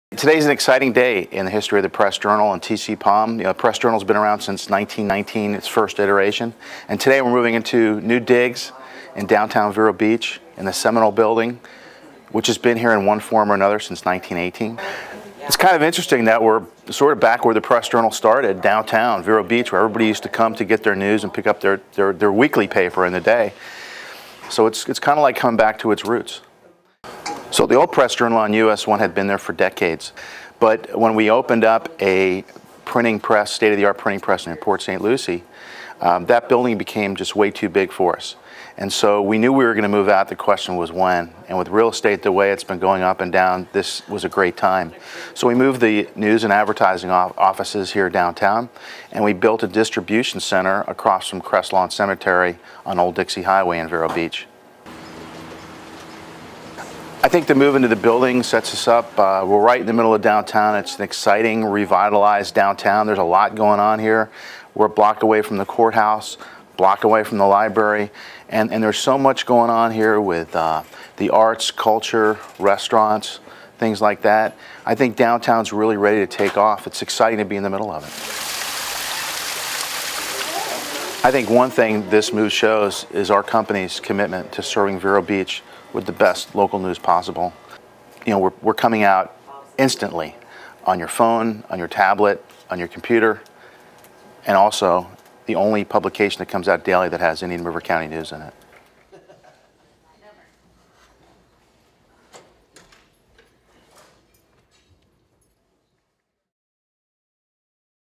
Indian River Press Journal returns to downtown Vero Beach
Community Editor Laurence Reisman talks about the Indian River Press Journal's move to downtown Vero Beach on the first day in the publication's new office.